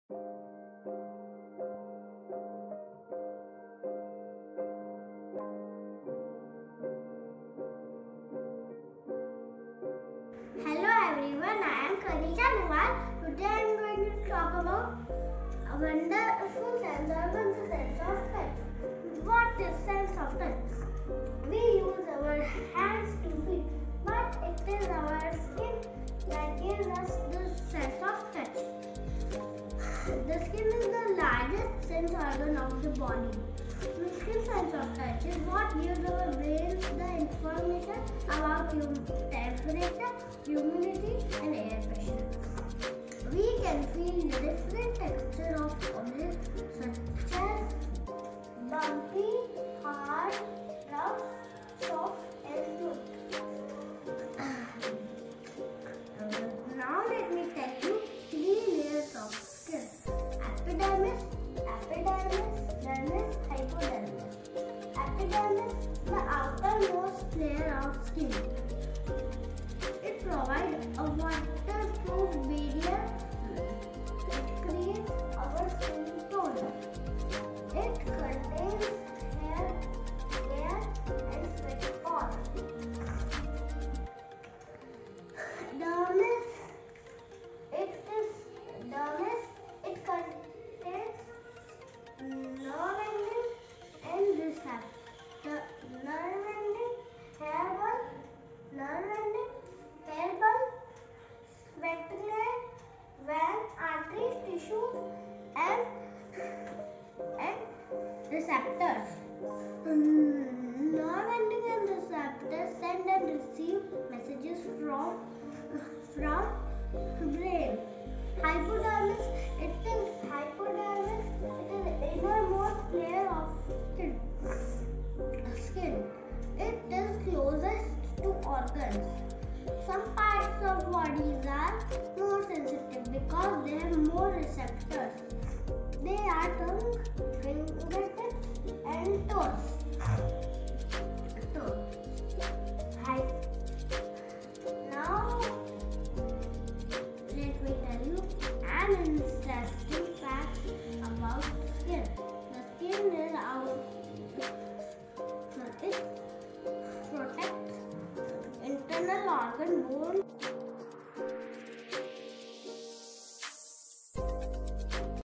Sense organ skin | Kindergarten science
Kanisha explains about our sense organ skin